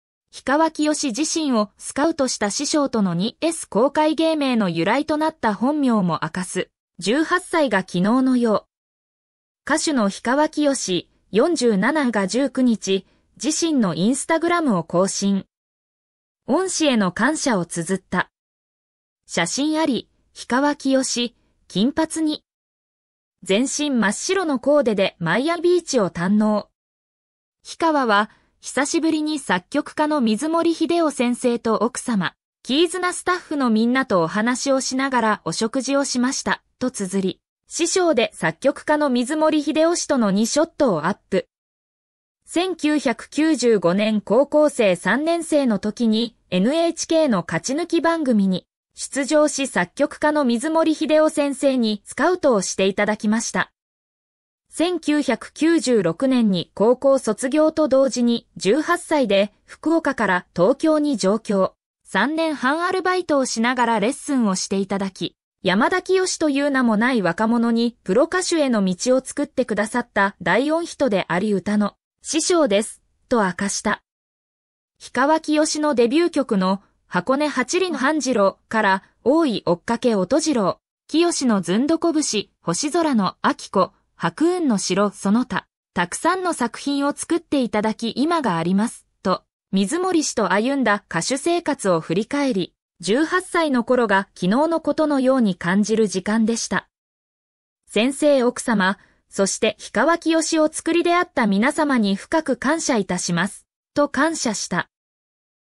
氷川きよし、師匠との感動2ショット公開！芸名の由来となった本名も初告白！
氷川きよしが、自身をスカウトした師匠との2ショットを公開！デビューのきっかけや、芸名「氷川きよし」の由来となった本名も明かし、18歳だった当時の思い出に浸る感動の瞬間をお届けします。ファン必見のエピソードとともに、彼の原点に迫る内容をお楽しみください！